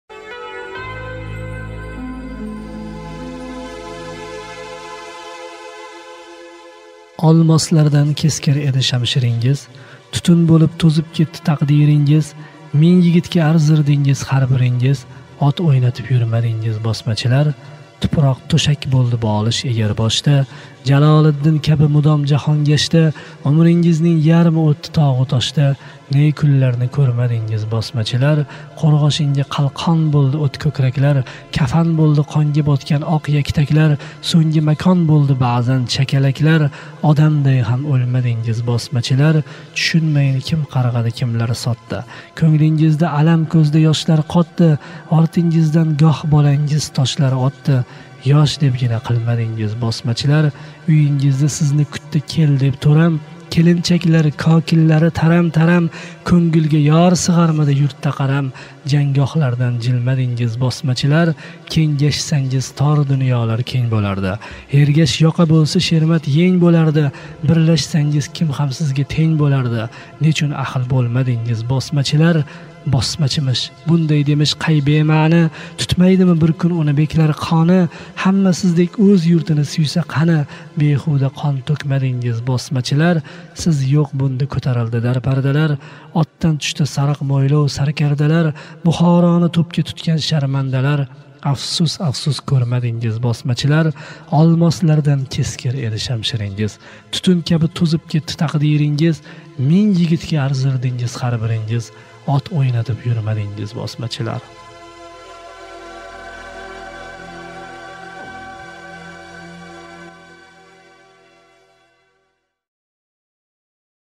Hayrulla Hamidov-She'r [BOSMACHILAR]HD
Sovet tarixi o'tmishda ozodlik harakati uchun kurashgan ota-bobolarimizni "bosmachilar"deb atagan. Aslida esa ular ozodlik kurashchilari bo'lishgan.Biz "bosmachilar" bilan faxrlanamiz!